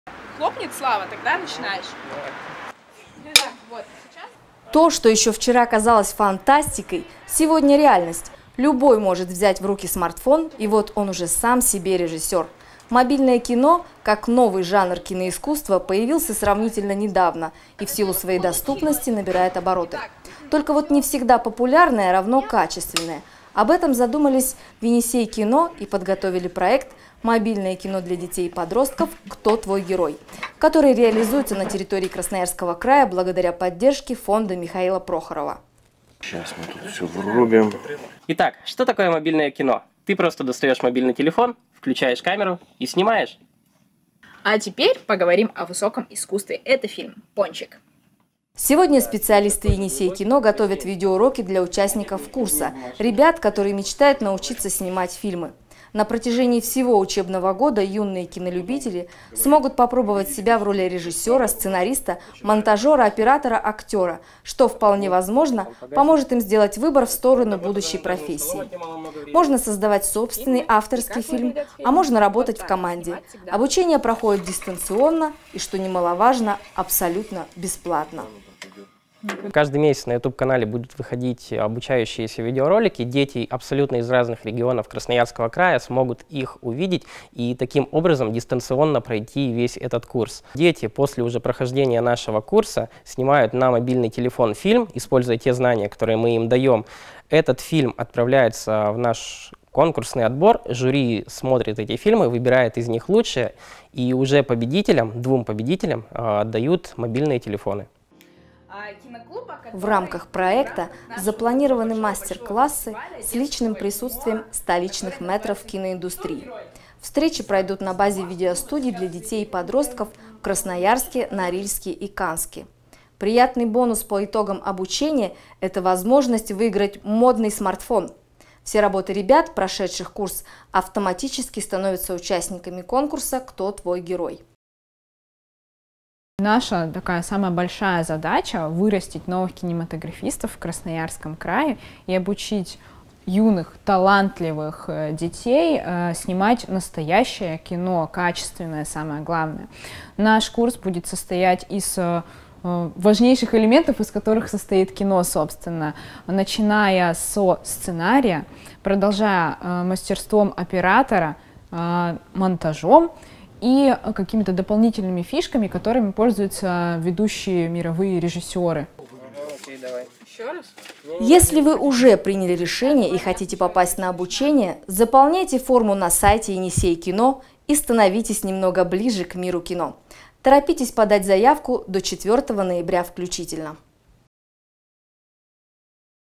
Репортаж о проекте "Мобильное кино: Кто твой герой?"
Совсем недавно "Енисей кино" при поддержке Фонда Михаила Прохорова дали старт масштабному проекту "Мобильное кино для детей и подростков: Кто твой герой?", в рамках которого дети со всего Красноярского края смогут поближе познакомиться с миром кино.

В конце каждого месяца с сентября по апрель мы будем публиковать обучающие видеоуроки, посмотреть которые смогут все желающие. А вот принять участие в конкурсе мобильных фильмов смогут только зарегистрированные на курсе.

Первое обучающее видео выйдет уже совсем скоро, а пока посмотрите репортаж о том, как оно создавалось.